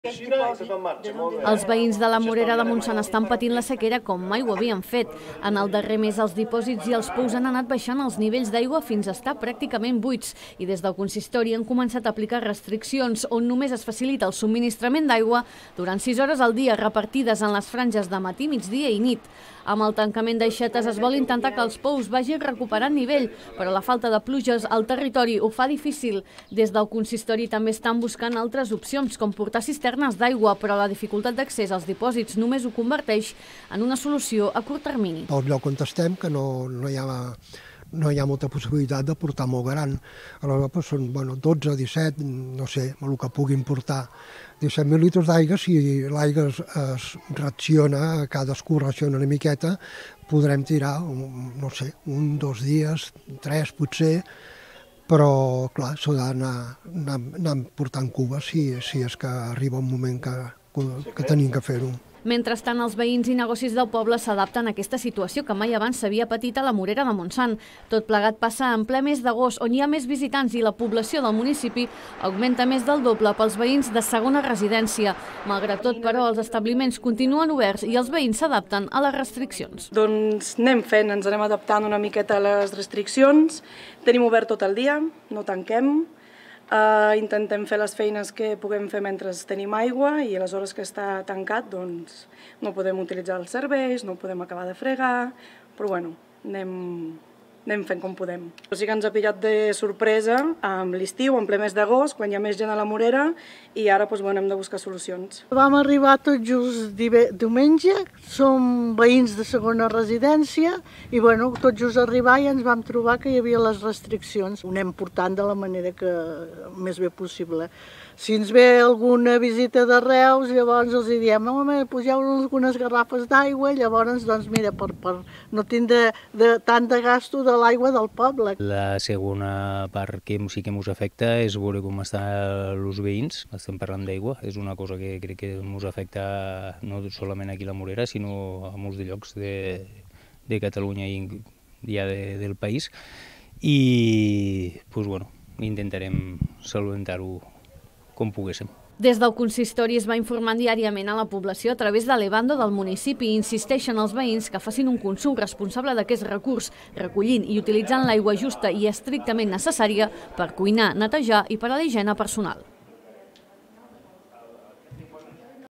La Morera de Montsant pateix les conseqüències de la sequera
El municipi ha vist com els seus pous i dipòsits d’aigua s’han anat assecant en el darrer mes, fet pel qual el consistori ha començat a fer talls en el subministrament d’aigua mentre no es trobi una solució

📺 La televisió de proximitat, la veu del territori, mira't!